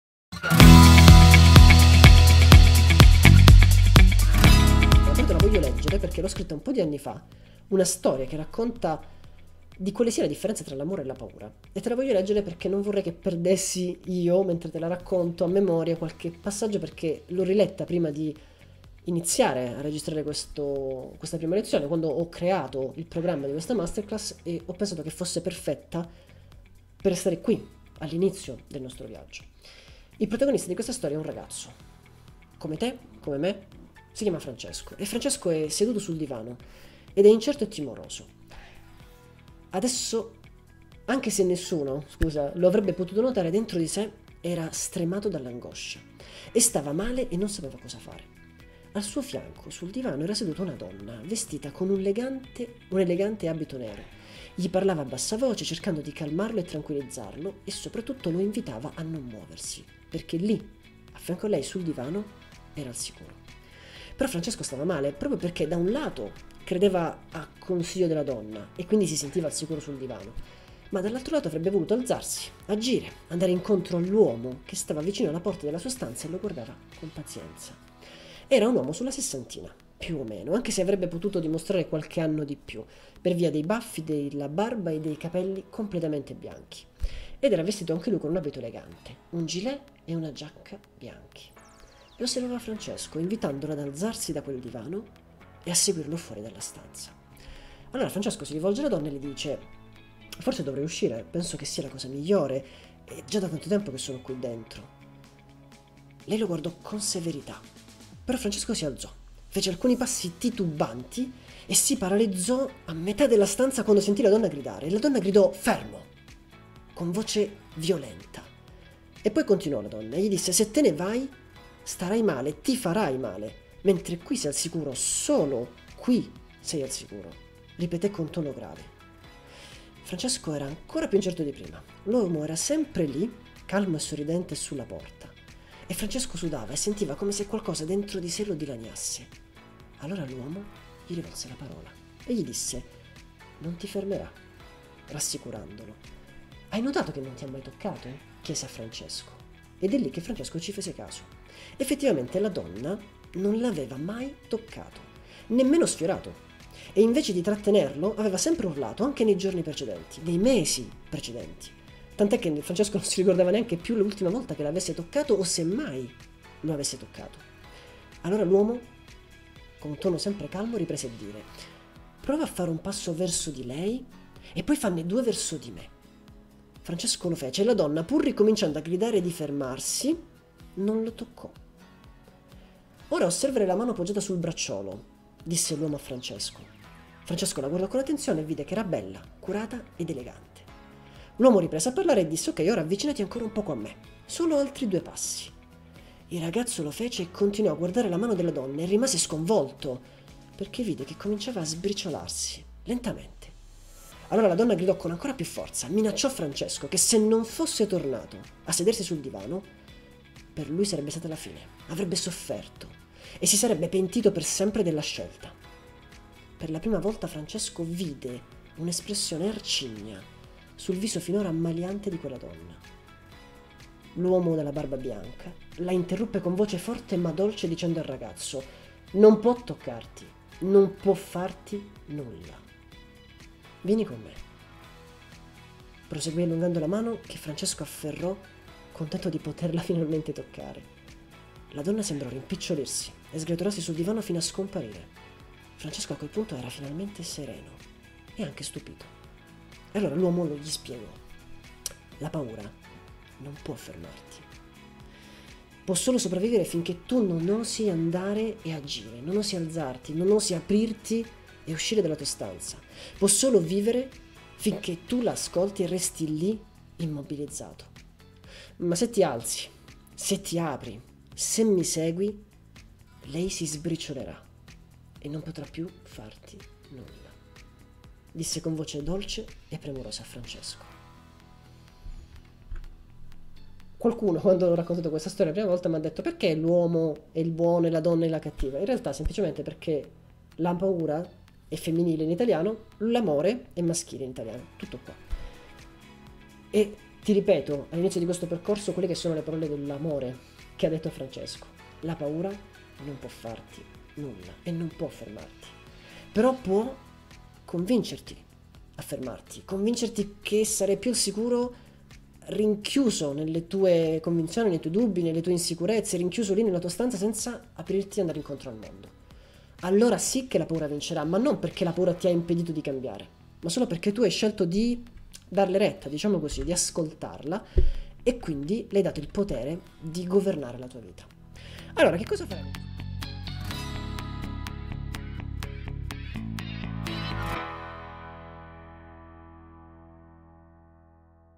Amore o Paura? Come liberarti dalle Tue Paure
Questo video è tratto dalla Masterclass della Scuola di Indipendenza Emotiva. Scopri di più su

Cosa può farti la paura?
La paura può paralizzarti, farti vivere una vita "sospesa" dove sei immobile mentre perdi occasioni, momenti preziosi, opportunità, anni che non torneranno mai più.

Anche Francesco era immobilizzato dalla paura fino a quando non capì come liberarsi delle sue paure.

In questo video ti racconto la storia di come Francesco ha imparato a sgretolare ogni paura.